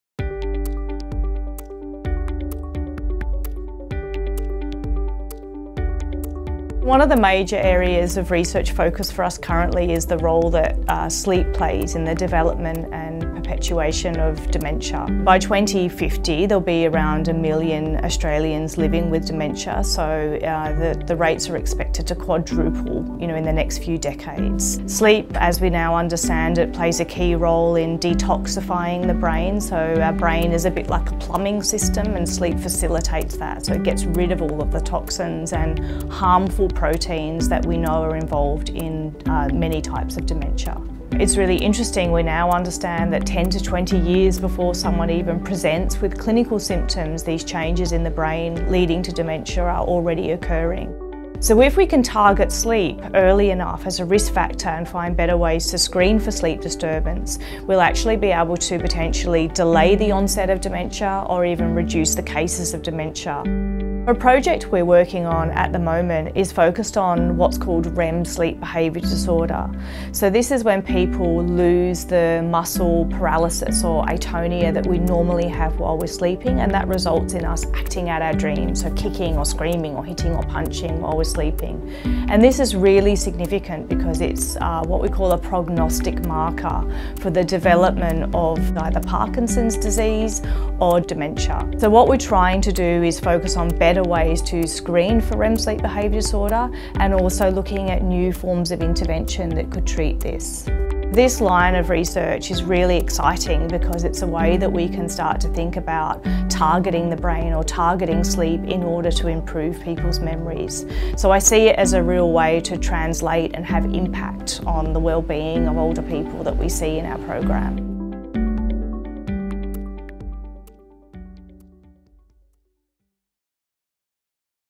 Connection between sleep and dementia
Researchers from the University of Sydney Brain and Mind Centre and the Woolcock Institute of Medical Research are looking into the connection between sleep and dementia to determine possible treatment or prevention.